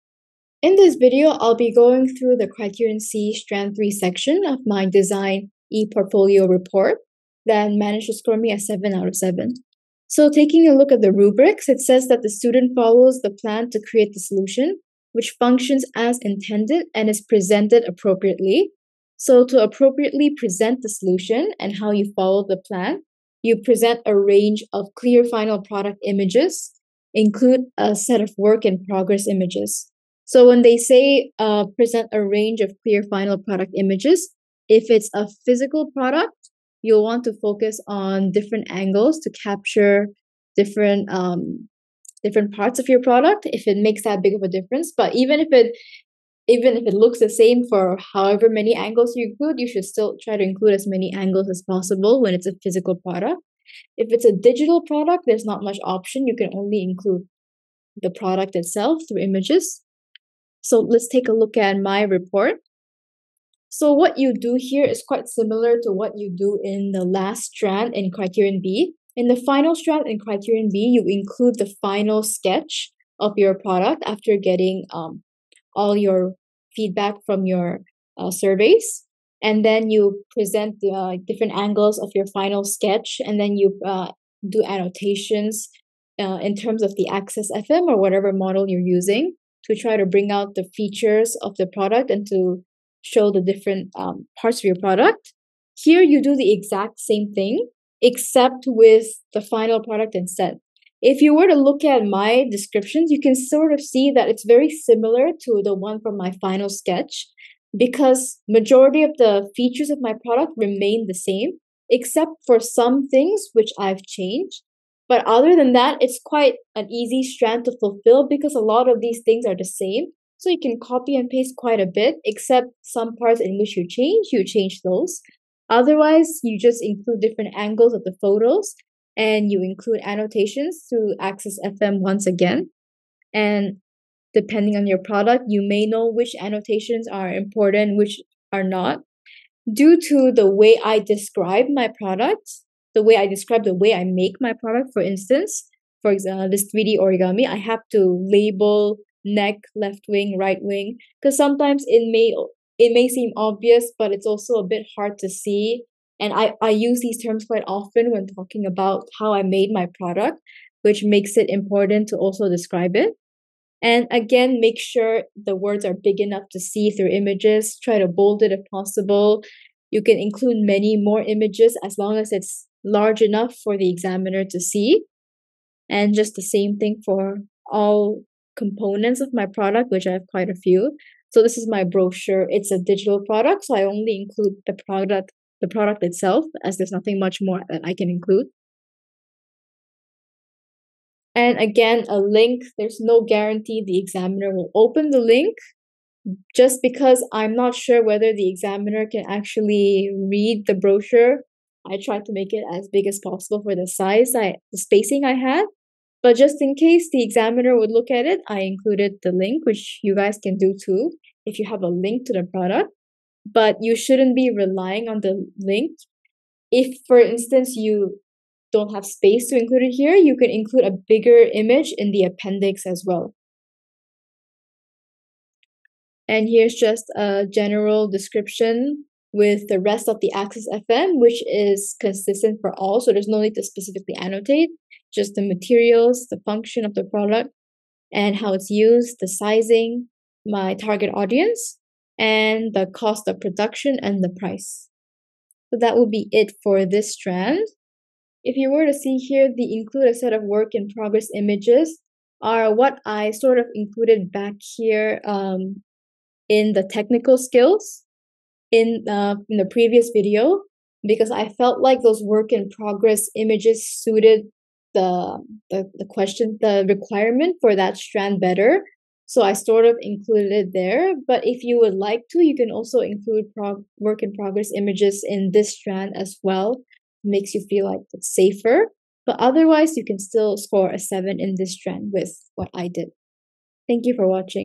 IB MYP Design ePortfolio Report: Criterion C Strand iii Walkthrough
My Design e-Portfolio tackles the issue of a lack of international mindedness present in my school community, in which I created a 3D origami Japanese good luck ornament in order to share aspects of Japanese culture, promoting International Mindedness in the process.

In this video, I walk through the Criterion C strand iii section of my Design report, breaking down the different aspects of my report as well as my thought process behind them. I introduce the way I structured my report, going into detail on the way I incorporated the Design rubrics into my report.

I hope the video would be of use to MYP students struggling with their Design ePortfolio. Best of luck!